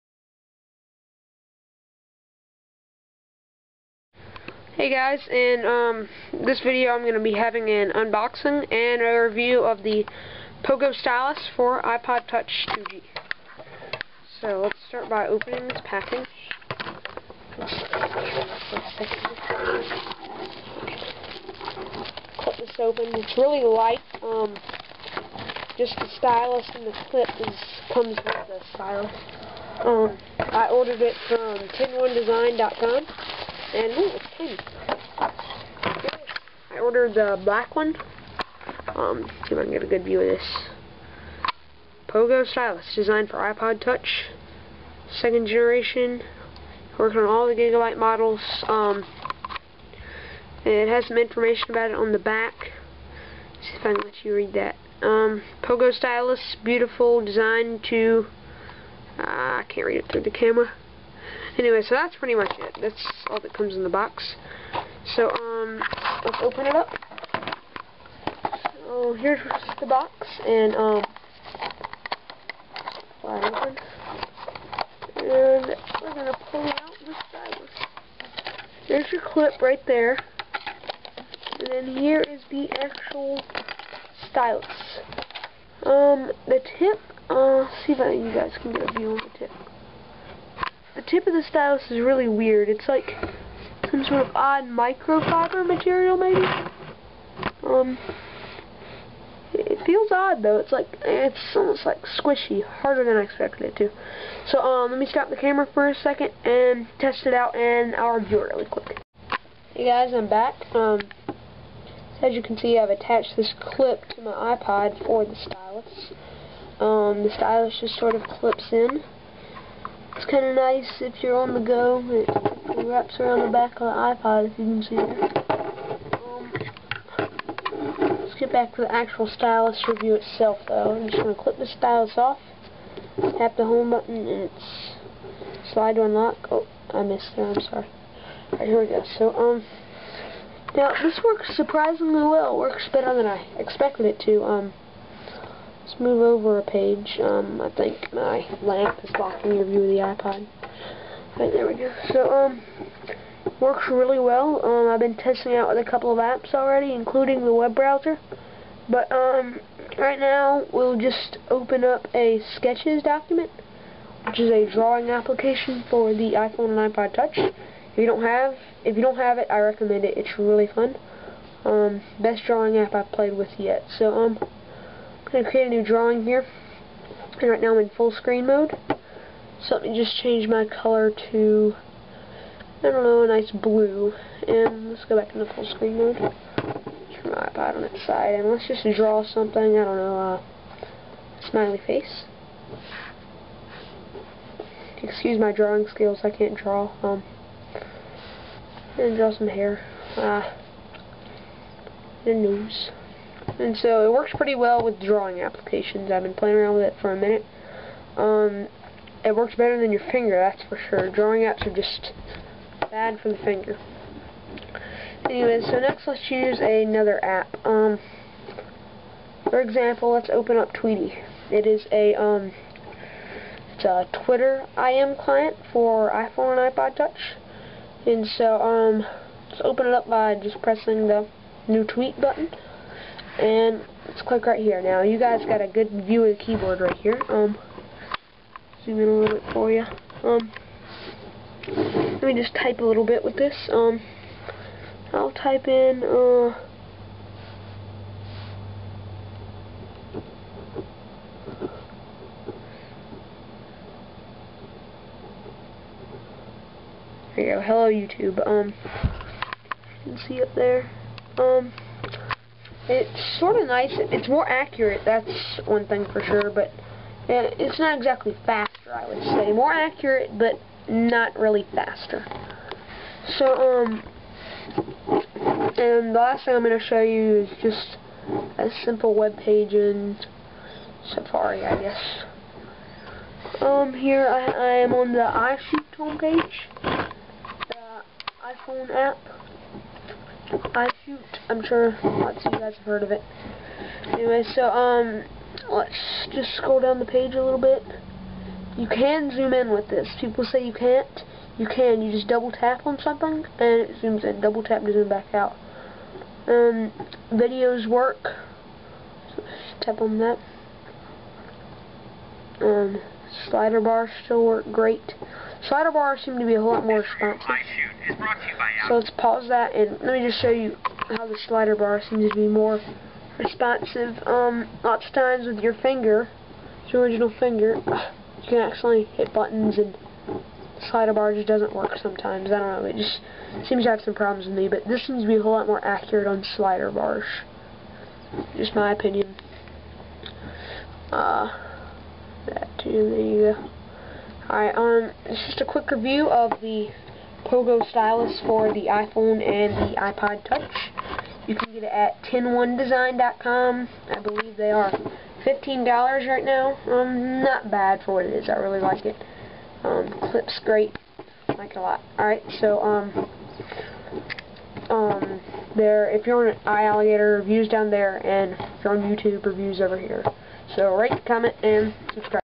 Pogo Stylus, unboxing and review
Review of the pogo stylus for the iphone and ipod touch